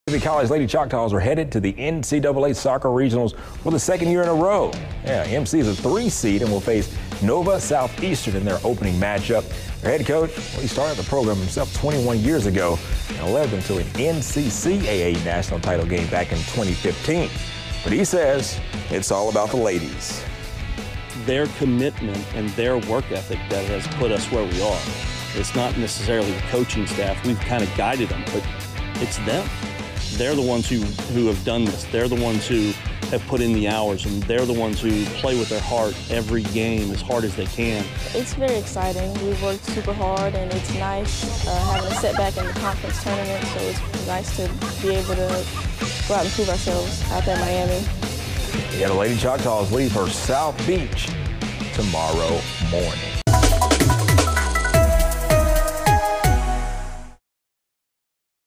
Lady Choctaws headed to South Beach
Mississippi College will play in NCAA Regionals (soccer) for the second year in a row.